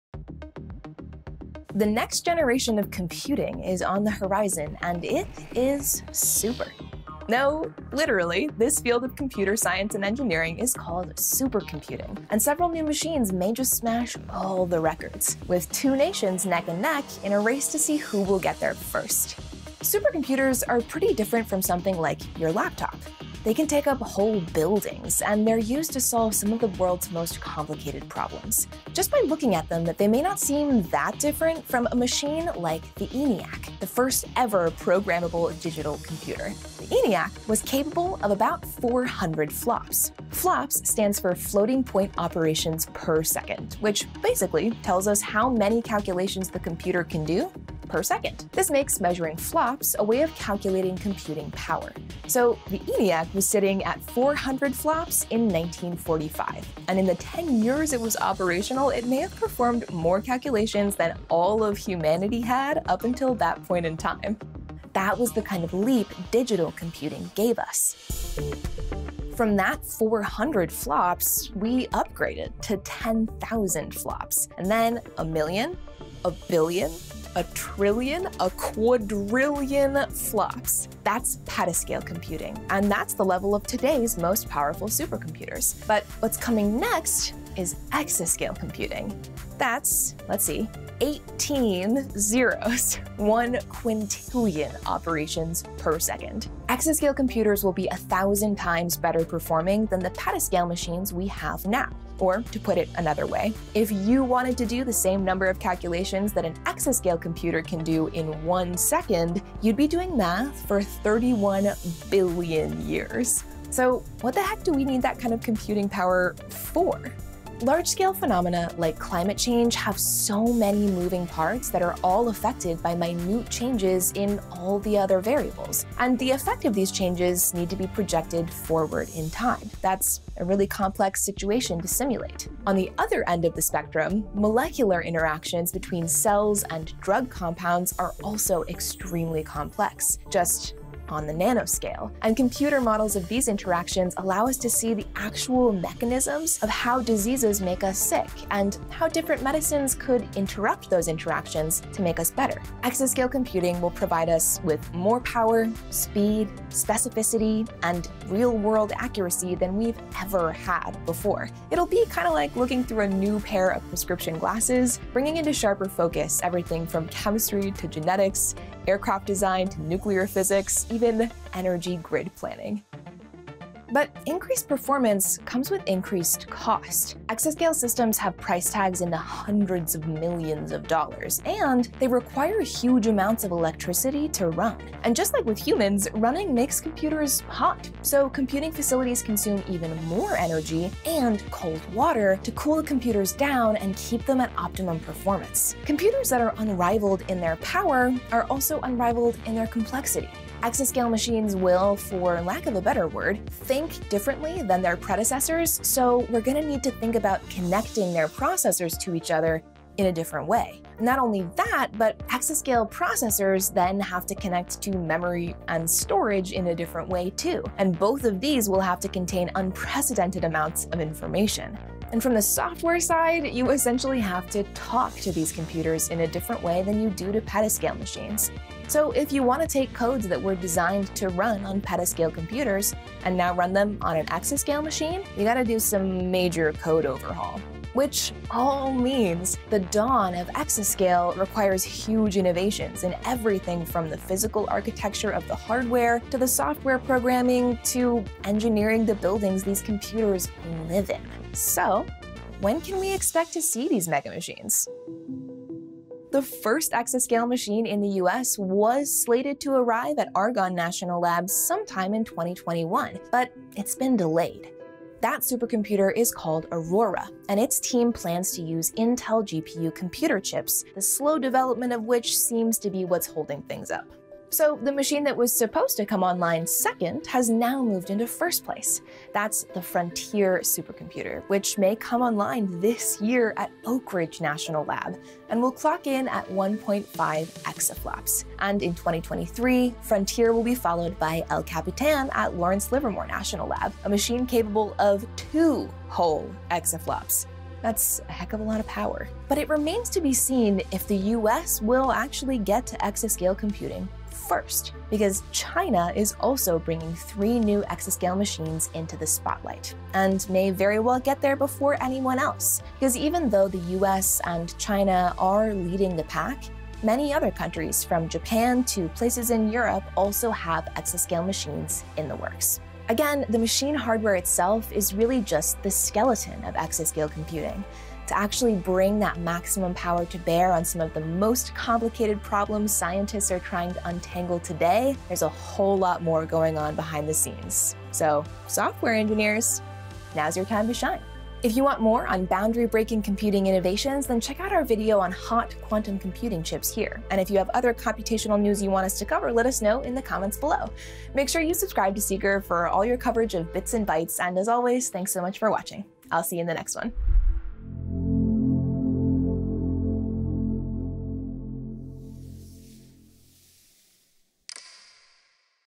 The World’s Most Powerful Supercomputer Is Almost Here
The next generation of computing is on the horizon, and several new machines may just smash all the records...with two nations neck and neck in a race to get there first.

The ENIAC was capable of about 400 FLOPS. FLOPS stands for floating-point operations per second, which basically tells us how many calculations the computer can do per second. This makes measuring FLOPS a way of calculating computing power.

So, the ENIAC was sitting at 400 FLOPS in 1945, and in the ten years it was operational, it may have performed more calculations than all of humanity had up until that point in time—that was the kind of leap digital computing gave us. From that 400 FLOPS we upgraded to 10,000 FLOPS, and then a million, a billion, a trillion, a quadrillion FLOPS. That’s petascale computing, and that’s the level of today’s most powerful supercomputers.

But what’s coming next is exascale computing. That’s zeroes. 1 quintillion operations per second. Exascale computers will be a thousand times better performing than the petascale machines we have now. Or, to put it another way, if you wanted to do the same number of calculations that an exascale computer can do in ONE second...you’d be doing math for over 31 billion years.

Will China Attain Exascale Supercomputing in 2020?
"Now the United States, China, Japan, and the European Union are all striving to reach the exaflop (1018 ) scale. The Chinese have claimed they will hit that mark in 2020. But they haven’t said so lately."

"So influential has ENIAC been since it was created that it changed electrical engineering and birthed new disciplines in the creative arts and sciences and computer science itself, said Thompson. 'Everyone’s touched by it,' he said. 'Anybody who reads an email.'"

"Imagine fitting the entire planet inside a computer. Using the world’s fastest supercomputers and big data models, climate scientists are creating computer models of the Earth’s air, water, and land systems at a global scale."